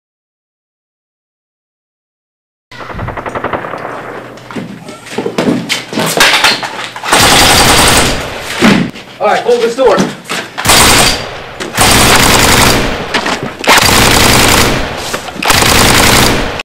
Zombie movie sneak peek
a short look at my upcoming zombie movie, we don't have a name for it yet so suggestions are welcome!